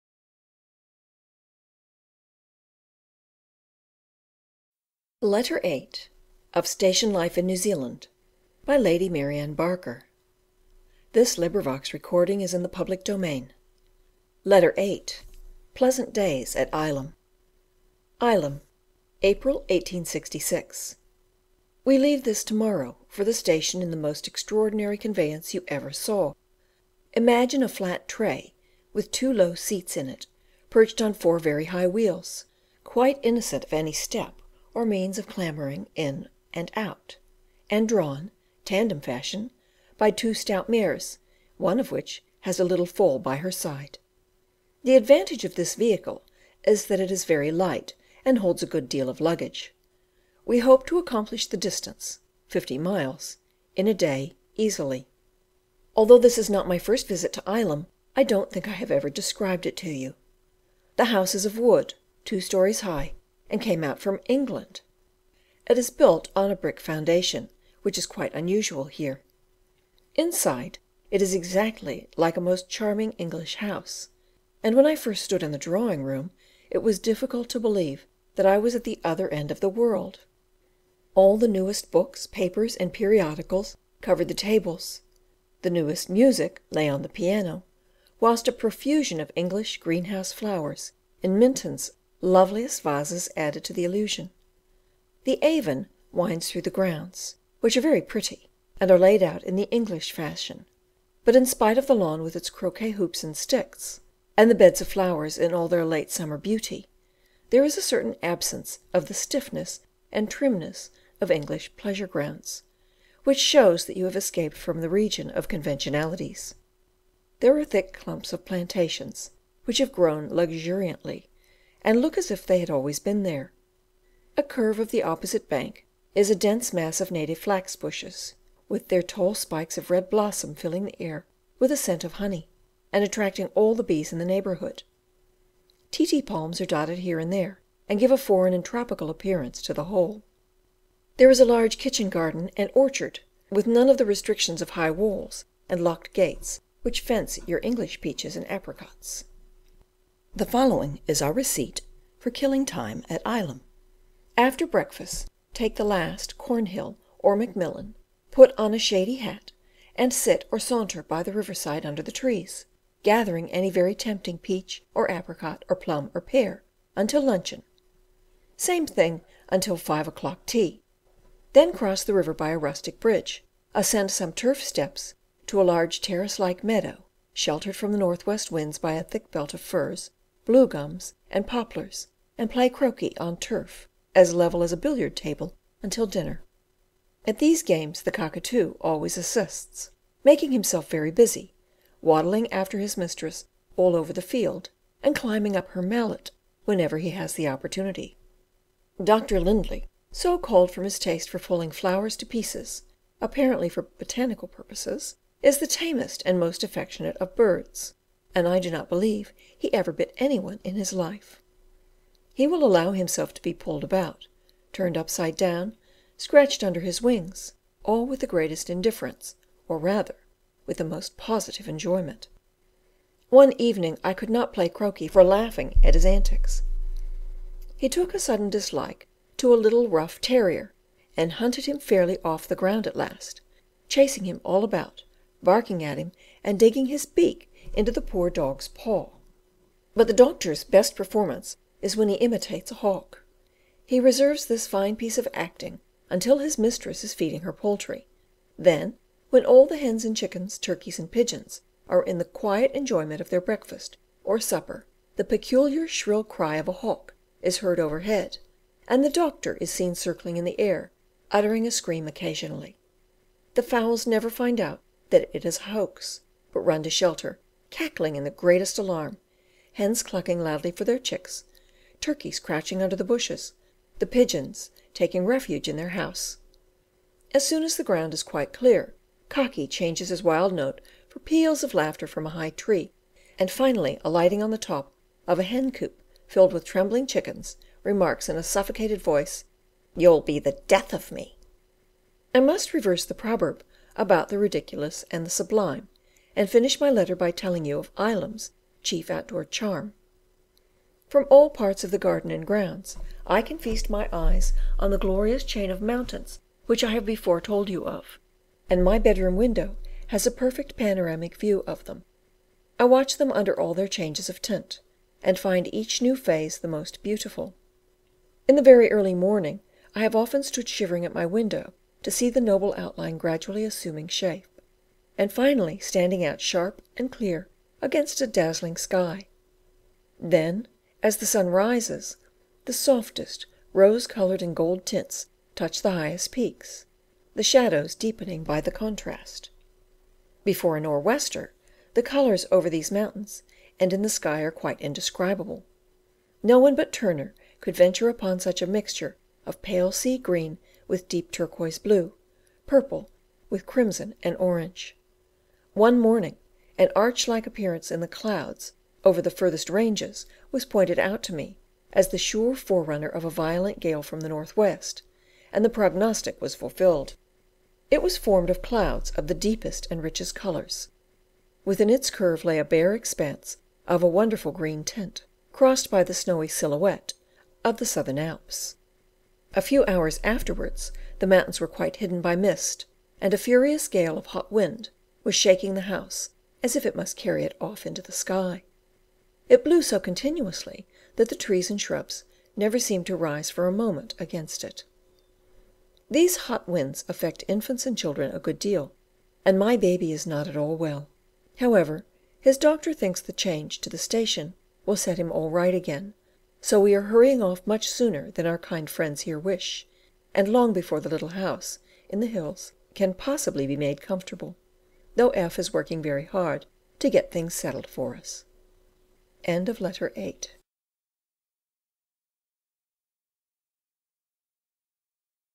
Station Life in New Zealand by Mary Anne Barker - Letter 8: Pleasant days at Ilam
LibriVox recording of Station Life in New Zealand by Lady Mary Anne Barker. Read by Gail Timmerman Vaughan.

Cover picture by Charles Decimus Barraud (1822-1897), Lake Coleridge Station, 1870. Copyright expired in U.S., Canada, EU. and all countries with author's life +70 yrs laws. Cover design by Gail Timmerman-Vaughan. This design is in the public domain.